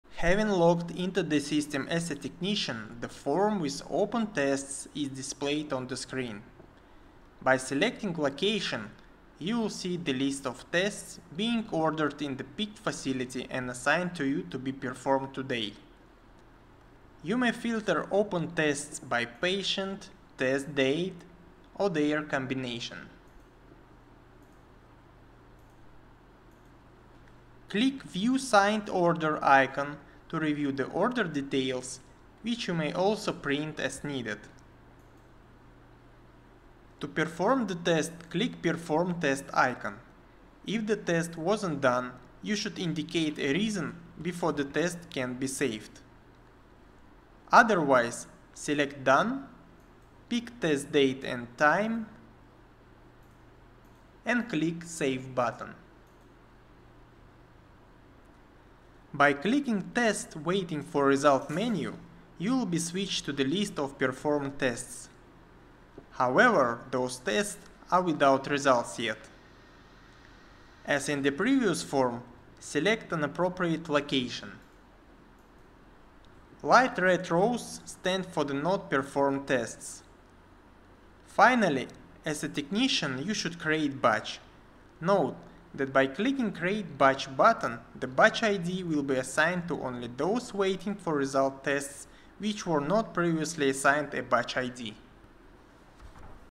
Technician workflow
The video contains instructions for users assigned 'Technician' role in Referral project.

Having logged into the system as a Technician the form with open tests is displayed on the screen. By selecting location, you will see the list of tests being ordered in the picked facility and assigned to you to be performed today. You may filter open tests by patient, test date or their combination.
Click ‘View Signed Order’ icon to review the order details, which you may also print as needed.
To perform the test click ‘Perform test icon’. If the test was not done you should indicate a reason before the test can be saved. Otherwise, select ‘Done’, pick test date and time and click ‘Save’ button.
By clicking ‘Tests, waiting for result’ menu you will be switched to the list of performed tests. However, those tests are w/o results yet. As in the previous form, select an appropriate Location. Light-red rows stand for the not performed tests.
Finally, as a technician you should create batch. Note that by clicking ‘Create butch’ button the butch ID will be assigned to only those waiting for result tests which were not previously assigned a butch ID.